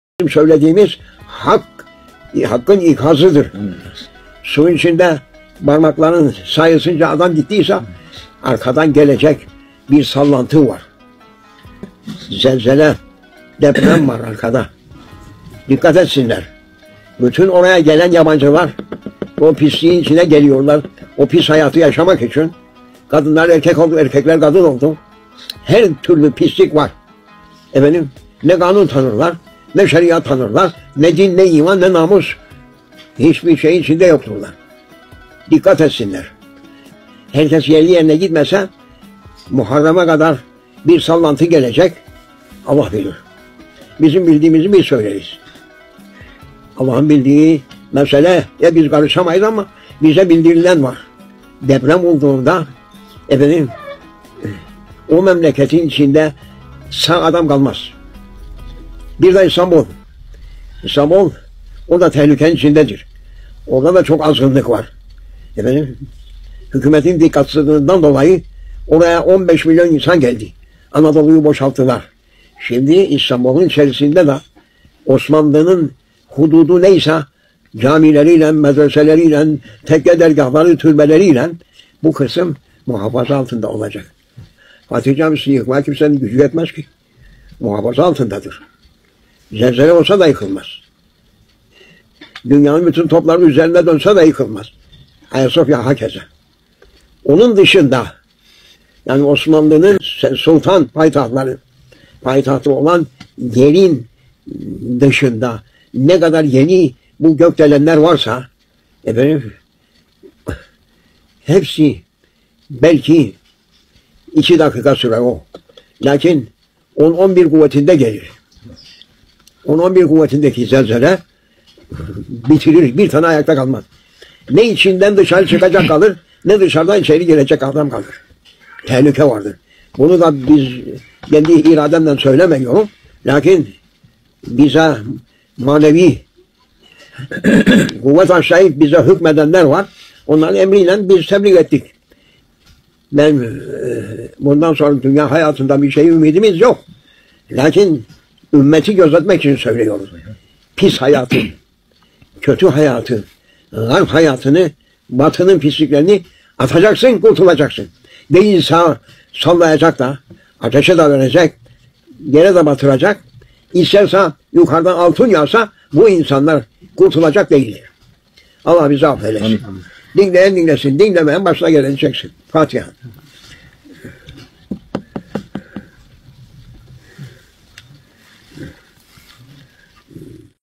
Şeyh Nazım efendi İstanbul Depremi, hakkındaki söylemleri
2014 yılında vefat etmiştir. mekanı cennet olsun ❤️🇹🇷